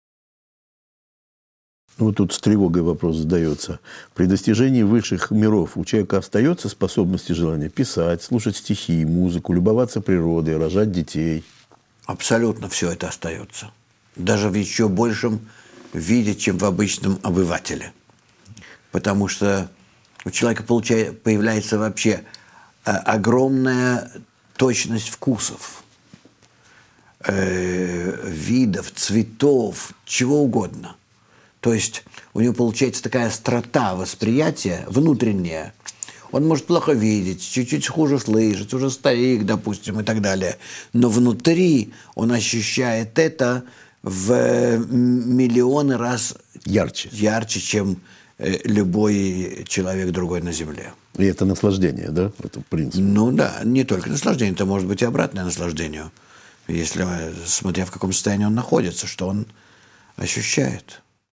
После постижения высшего мира
После постижения Высшего мира  человек не только не теряет  свои прежние желания и ощущения, но наоборот приобретает дополнительный орган восприятия. Через этот новый духовный орган он начинает воспринимать мир в миллионы раз шире, острее и ярче.